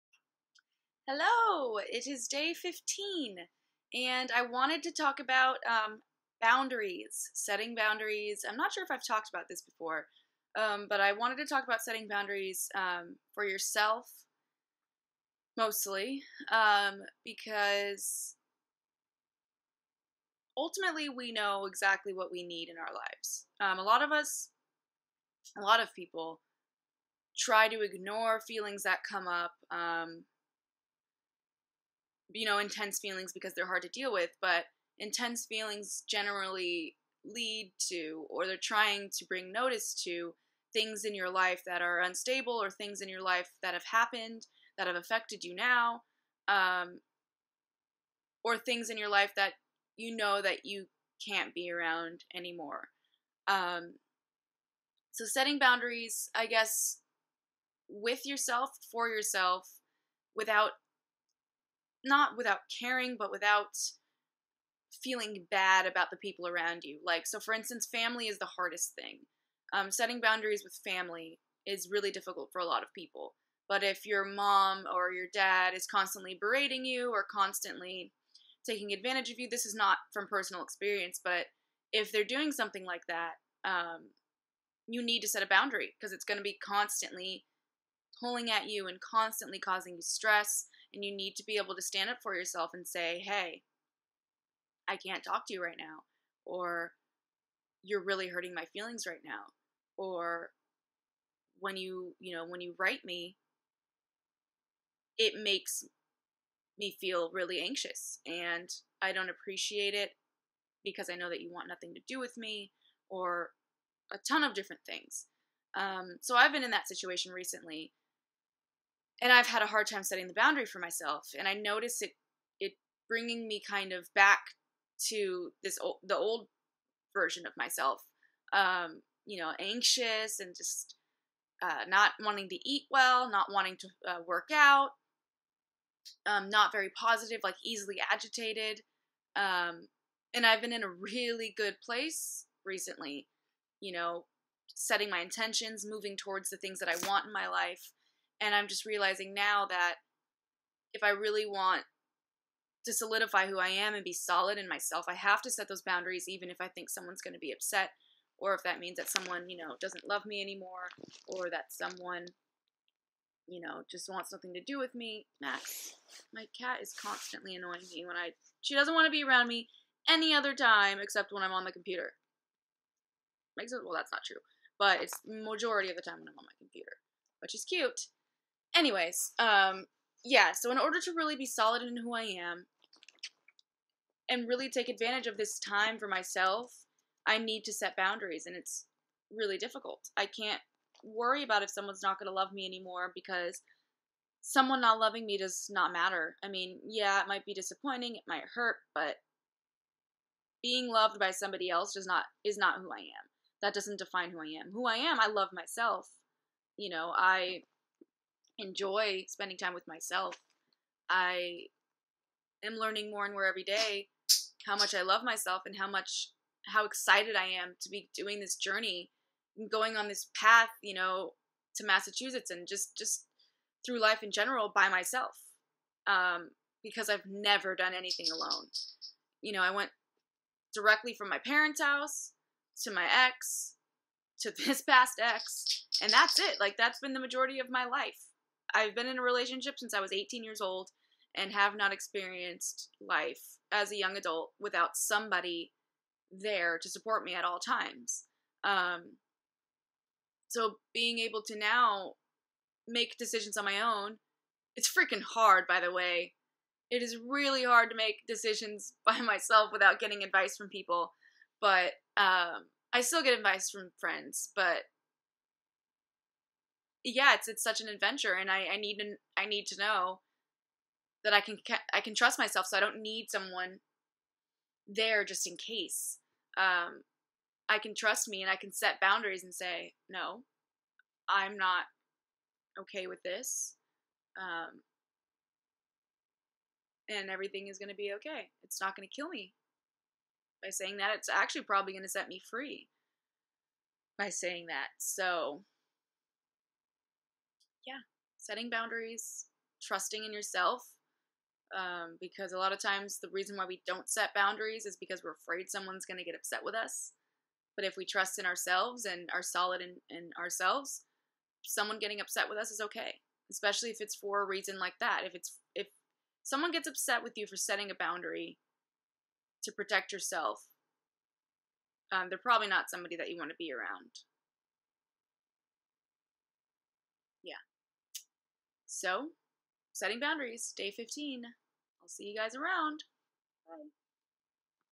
DAY #15 of 100 // SETTING BOUNDARIES
So excited to share my journey with you. 100 Days of self-reflection, self-love, and self-growth. We're in this together! Today I'm talking about setting healthy boundaries even if someone in your life doesn't like it. Sometimes we forget that we are allowed to put a stop to something we don't like, or that isn't healthy for us. Most of us have a hard time doing this with an ex, a family member, and sometimes even friends or acquaintances. But the reality is - we are in control of our loves and we are the only ones that can set boundaries.

People only do to you what you allow them to. And I'm not holier than thou, I've definitely taken advantage of this and have acted in completely unacceptable ways to people that I care for. We are all human, we all make mistakes. But we are still allowed to set boundaries. NO mistake makes you unworthy of happiness.

@babblingbmarie on snapchat!